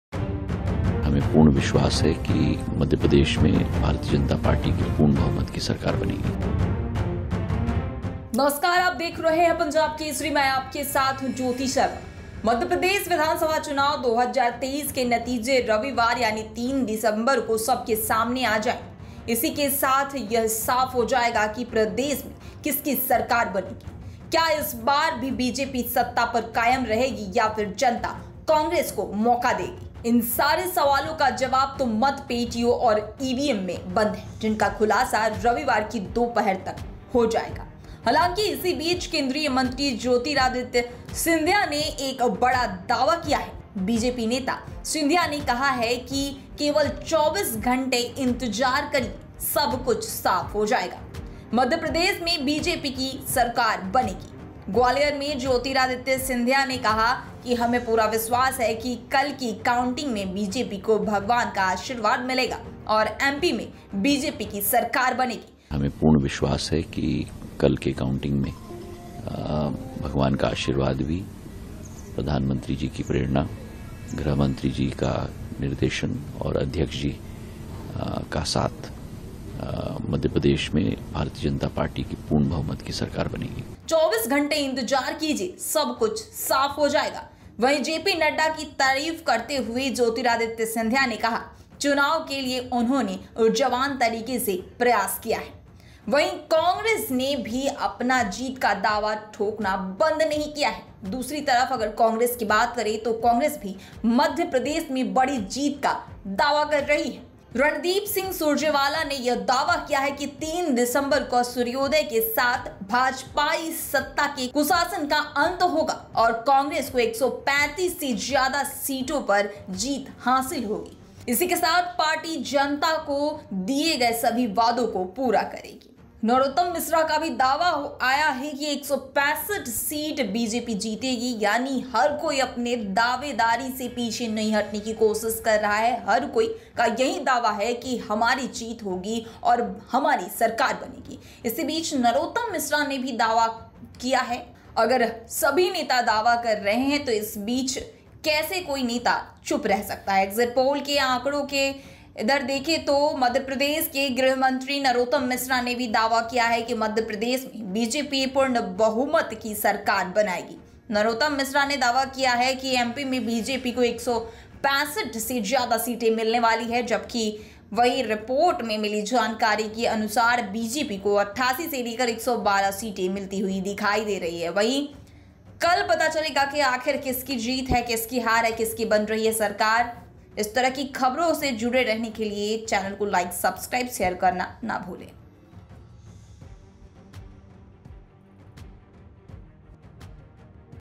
MP Election 2023:चुनाव के नतीजों को लेकर ज्योतिरादित्य सिंधिया का बड़ा दावा
In Gwalior, Jyotiraditya Scindia said that he has full confidence that God will bless the counting of votes and only BJP government will be formed in Madhya Pradesh.